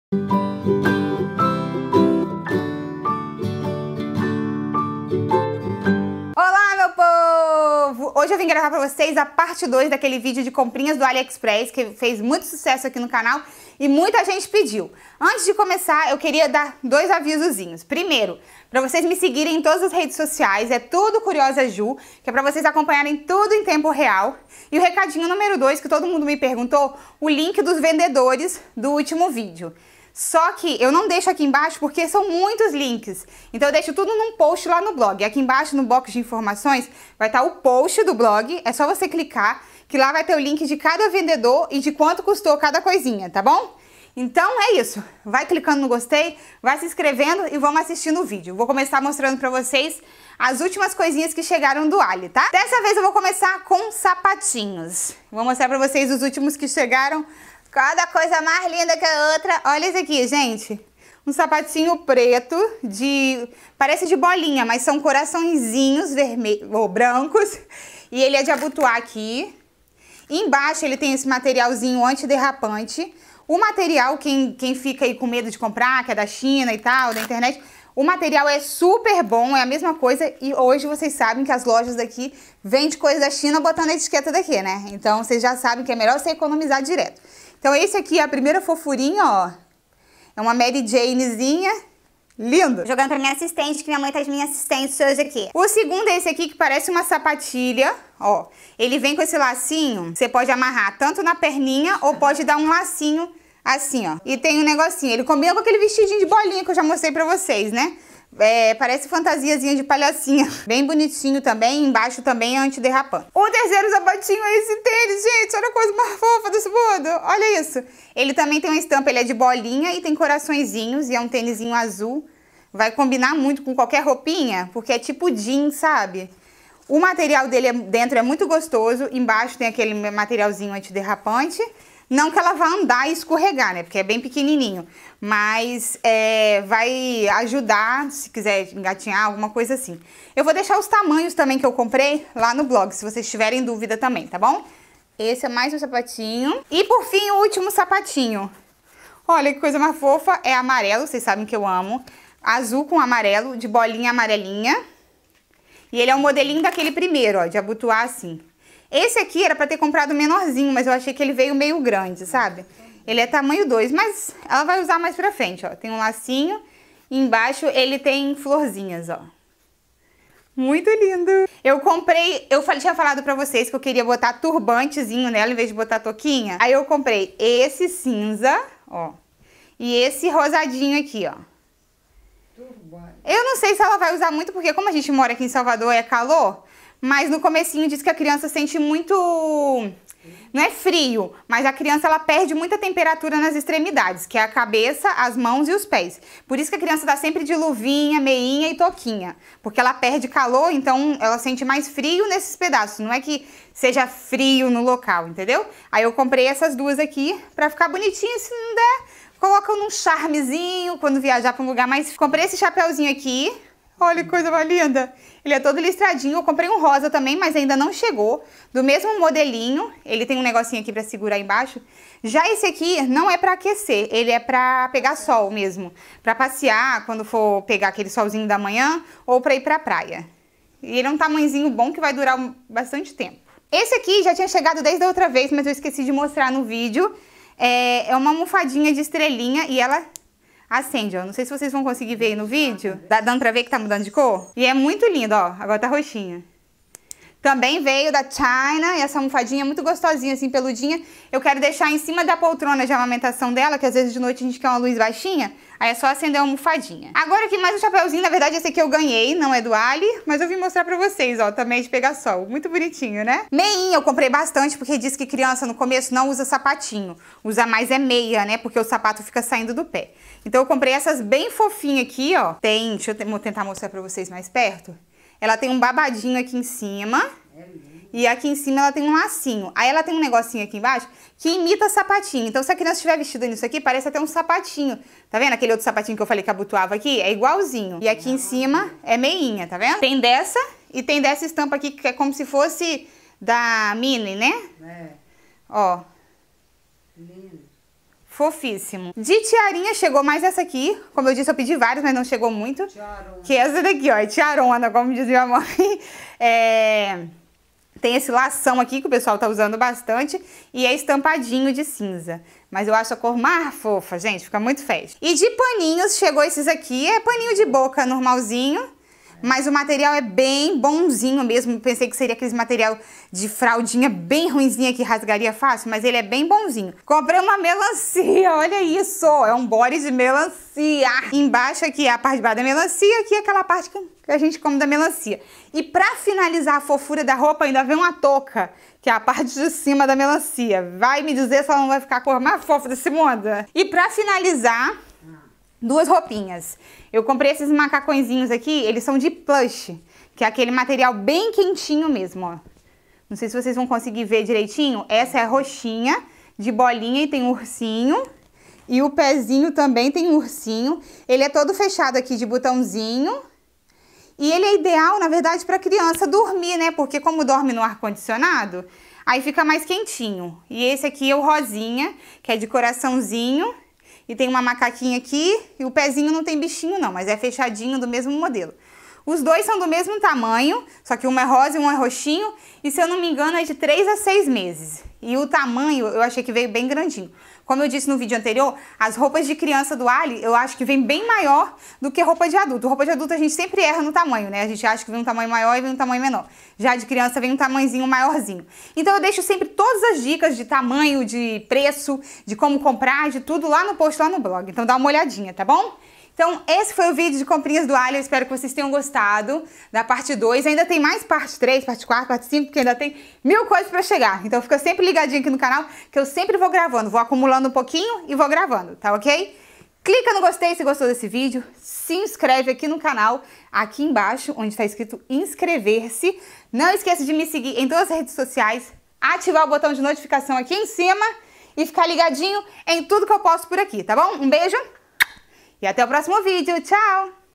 Enxoval de bebe no ALIEXPRESS - parte 2 | Comprinhas de bebe na China
Comprinhas para bebe no site da china Aliexpress - parte 2- linkd os produtos